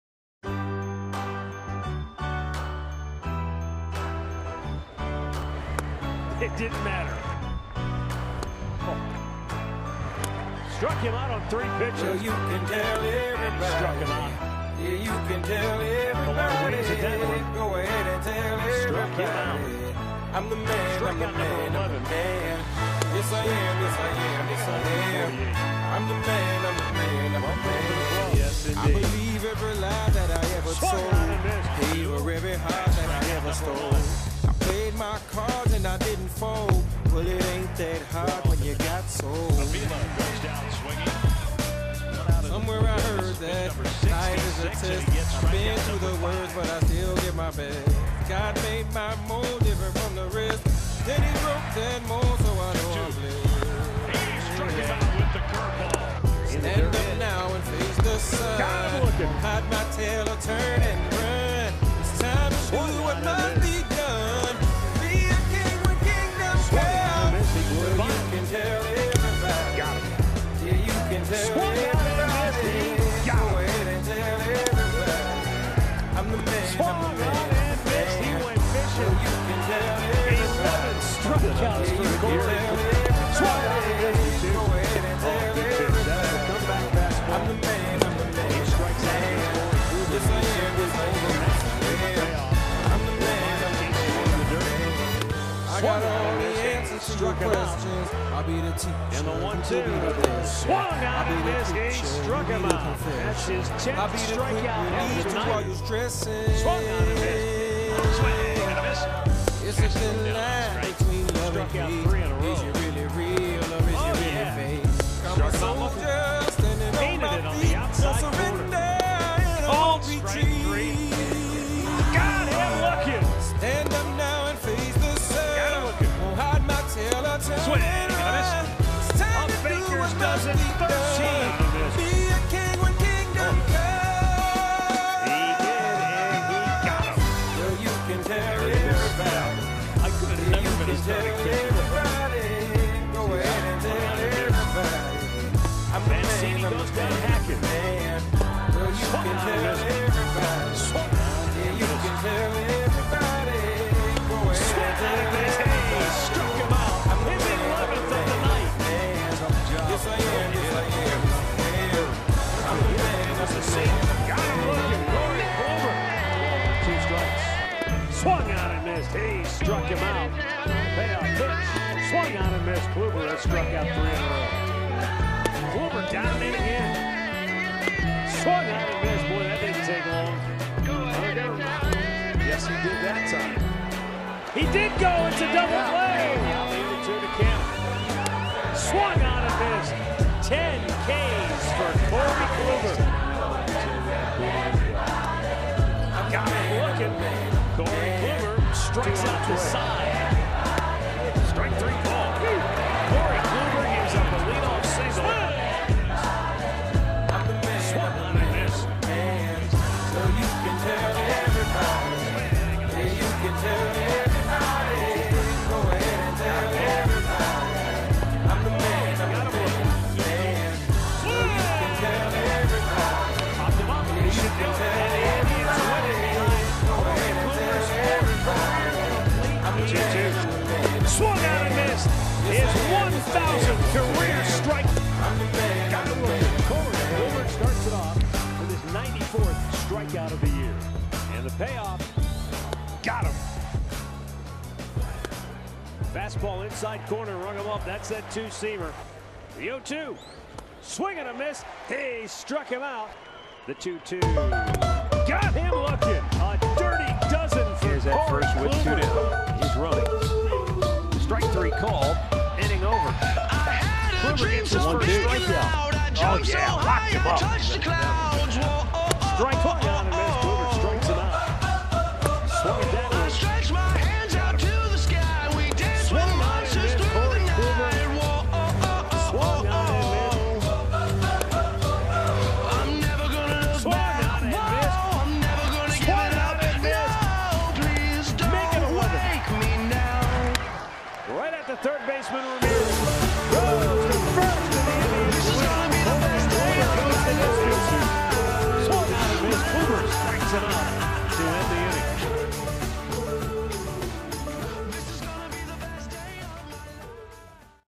Corey Kluber 2017 Highlights [HD]
Highlight video I created for Corey Kluber of the Cleveland Indians.

All videos are owned solely by Major League Baseball or MLB.com.

Corey Kluber Highlights